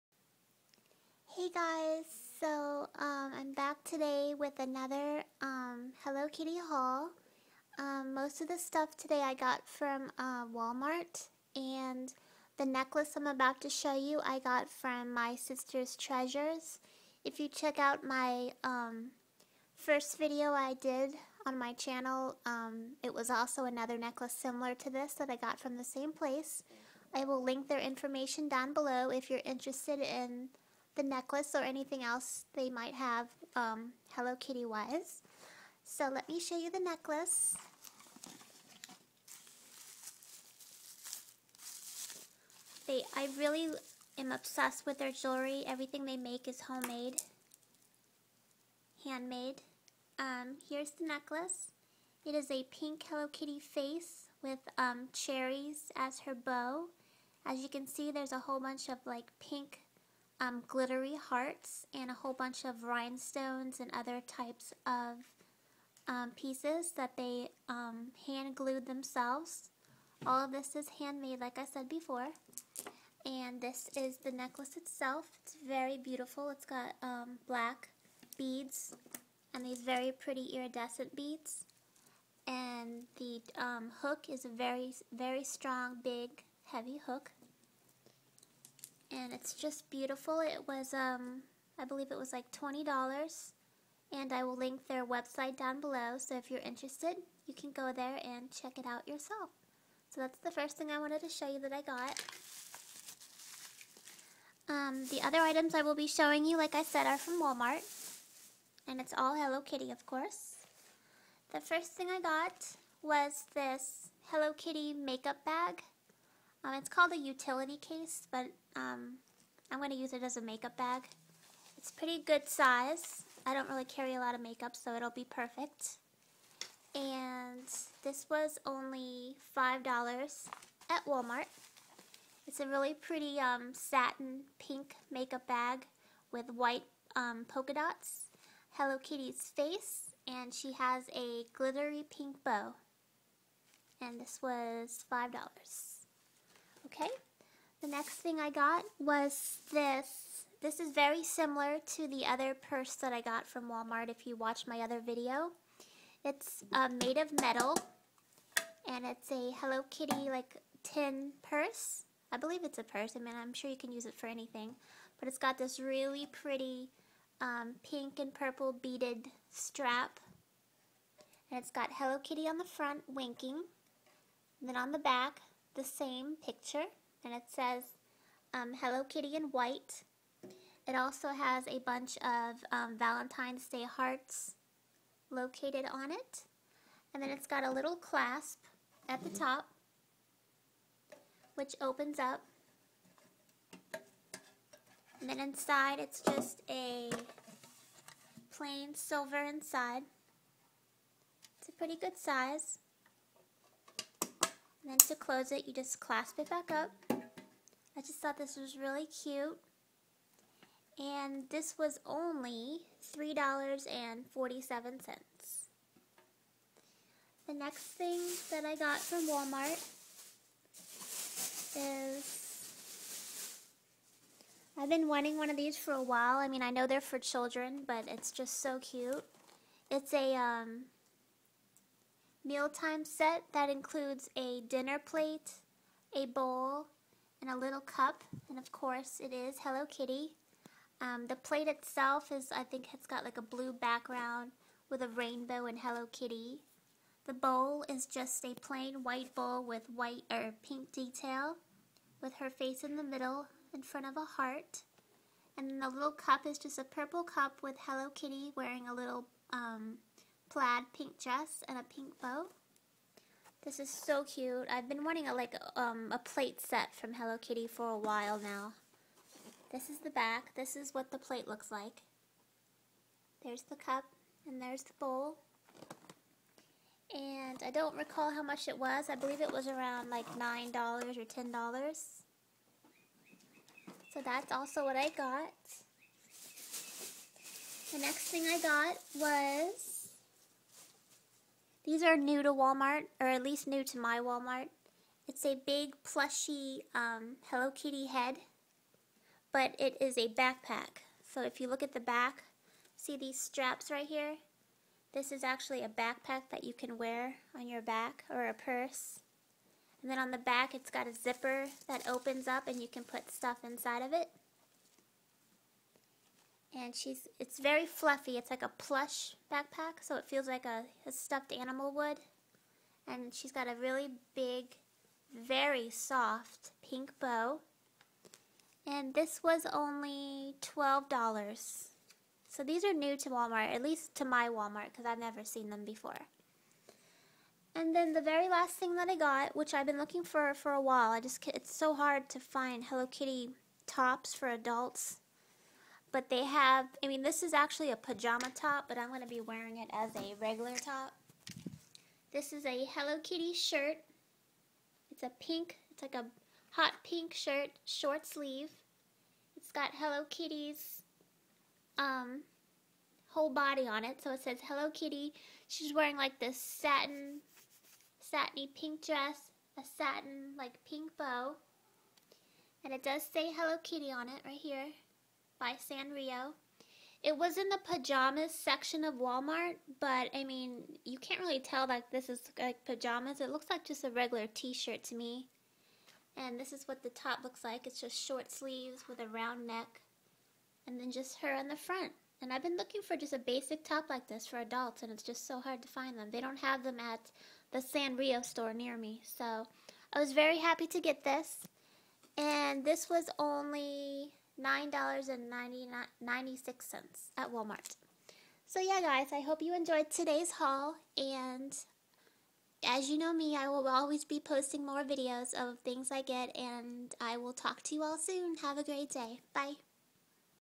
Hello Kitty Haul!!!
Cute Hello Kitty items I got from Walmart today! The necklace I got from My Sisters Treasure.